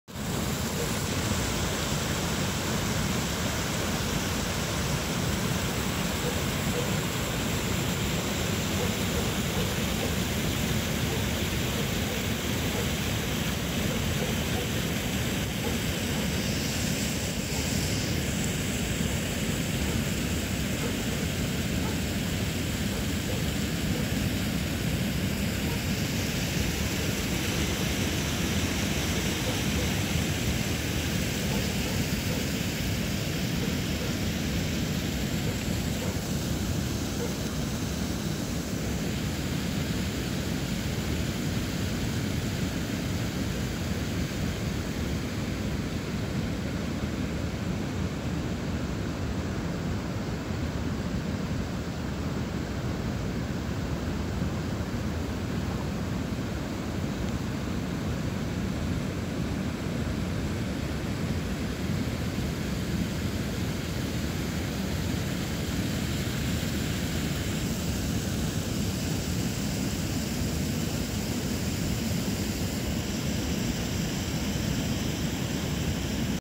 Krioklys per Ventos upę Kuldiga ( Venta waterfall )
Ventos slenkstis arba Ventos krioklys – slenkstis Ventos upėje Latvijoje, greta Kuldygos. Jei šis darinys laikytinas kriokliu, tuomet tai plačiausias krioklys Europoje. Plotis – 249 m, per pavasario potvynius – 270–275 m; vidutinis aukštis – 1,8-2,2 m, didžiausias – ~2,5 m. Vikipedija
Waterfall is a gorge on the Venta River in Latvia, near Kuldyga. If this formation can be considered a waterfall, then it is the widest waterfall in Europe. Width - 249 m, during spring floods - 270-275 m; average height - 1.8-2.2 m, maximum - ~2.5 m. Wikipedia